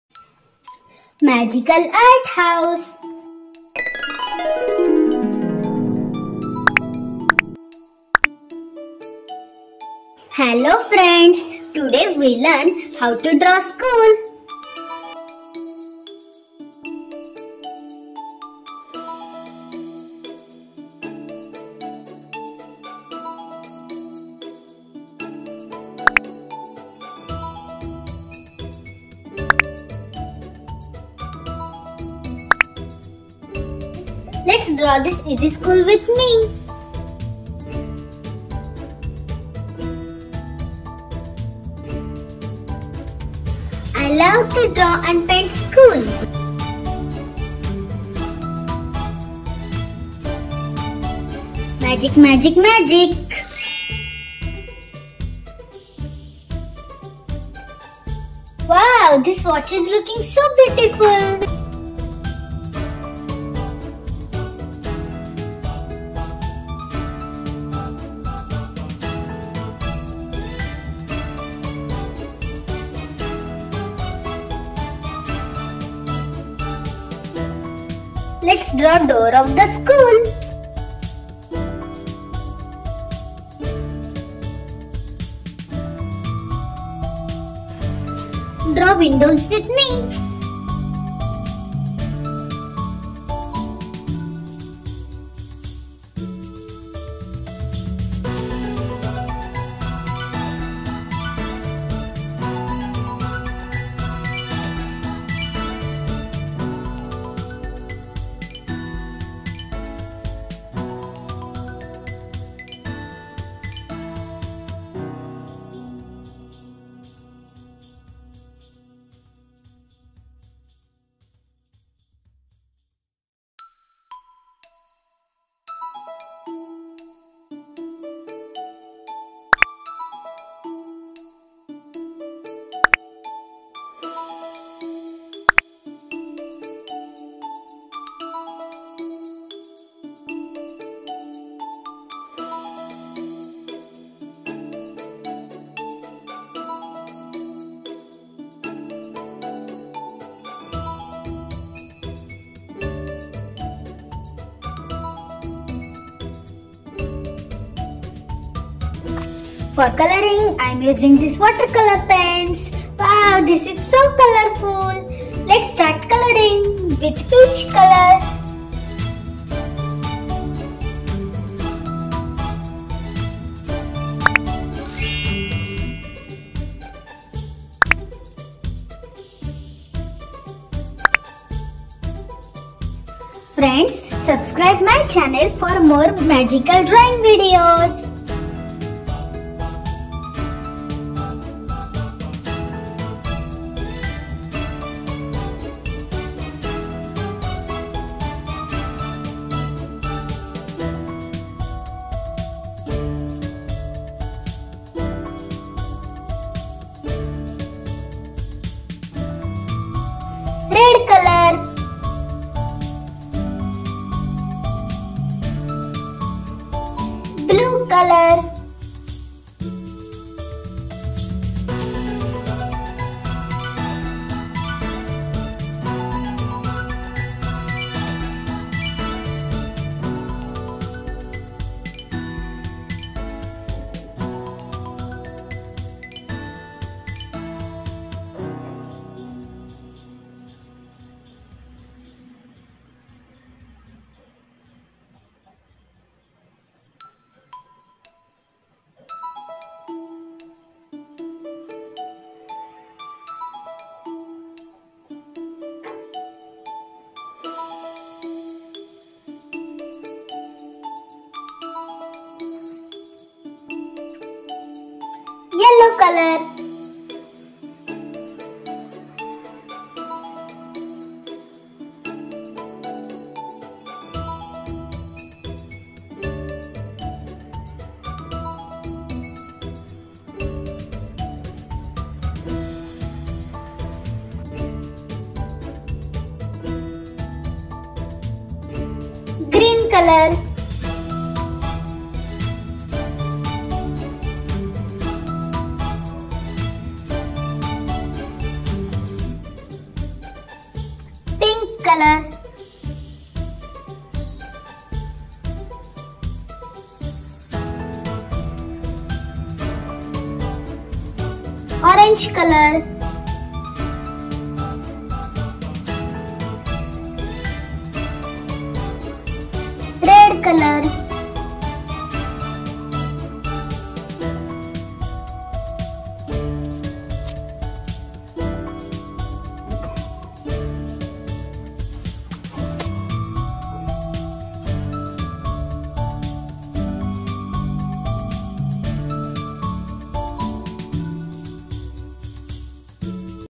School drawing for kids & Toddlers || Easy Drawing with MAGIC
Hello everyone
Todat i'm drawing a School
This is so beautiful.
I love drawing and painting.
Please try to draw and paint with me...
Learn Drawing Basics and Coloring for kids and Toddlers